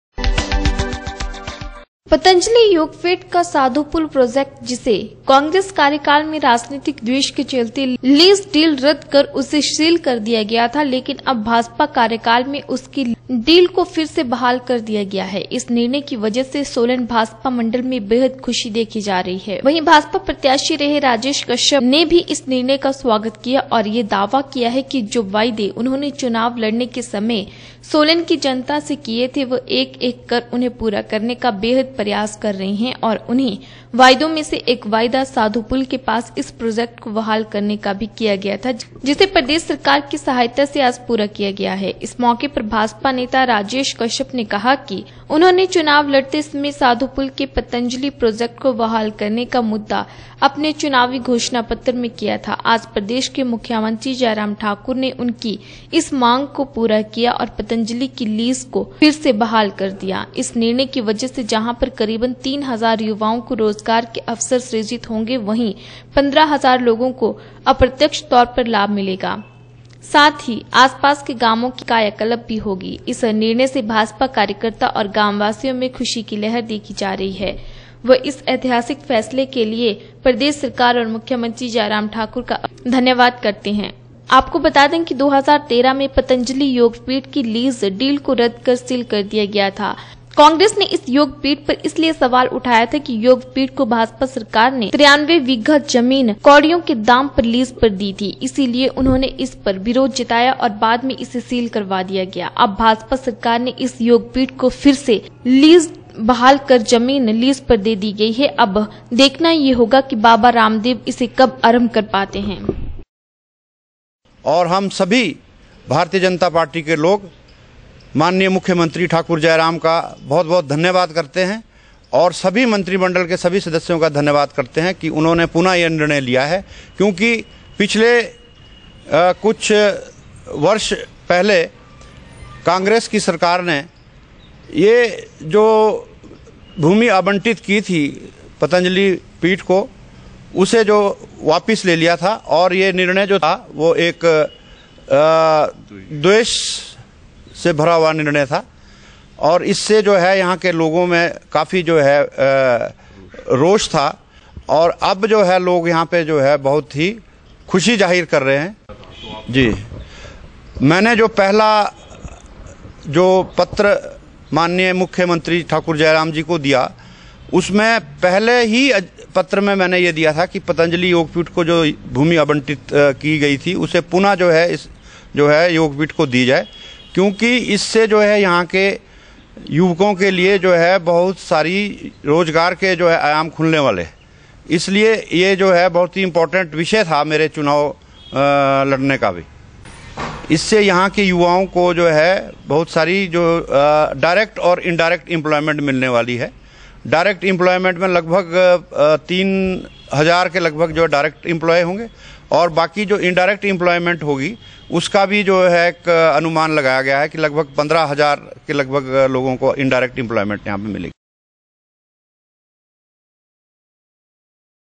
पतंजलि योगपीठ का साधुपुल प्रोजेक्ट जिसे कांग्रेस कार्यकाल में राजनैतिक द्वेष के चलते लीज़ डीड रद्द
पतंजलि योगपीठ का साधुपुल प्रोजेक्ट जिसे  कांग्रेस कार्यकाल में राजनैतिक द्वेष के चलते लीज़  डीड रद्द कर उसे सील कर दिया गया था लेकिन अब भाजपा कार्यकाल में उसकी लीज डीड को फिर से बहाल कर दिया है । इस निर्णय की वजह से सोलन भाजपा मंडल में बेहद खुशी देखी जा रही है वहीं भाजपा प्रत्याशी रहे राजेश कश्यप नेवी इस निर्णय का स्वागत किया और यह दावा किया कि जो वायदे उन्होंने चुनाव लड़ने के समय सोलंकी जनता को किए थे वह एक एक कर उन्हें पूरा करने का बेहद प्रयास कर रहे हैं और उन्हीं वायदों में से एक वायदा साधुपुल के इस प्रोजेक्ट को बहाल करने का भी किया गया था जिसे प्रदेश सरकार की सहायता से आज पूरा किया गया ।